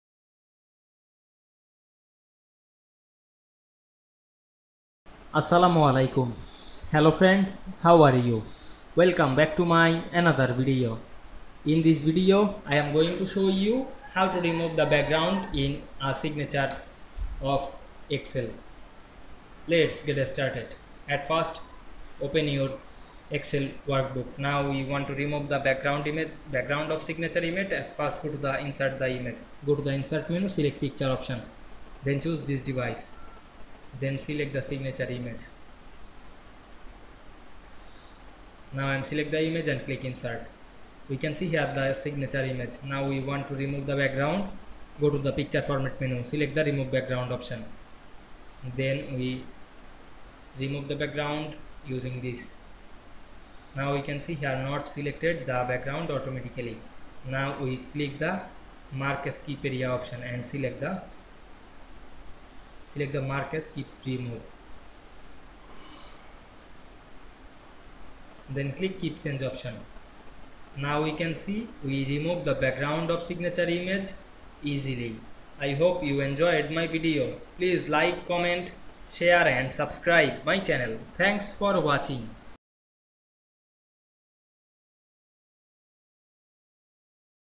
How to remove the background in a signature in Excel
Assalamu Walaikum,
In this video I will show you, How to remove the background in a signature in Excel.  Let's get started.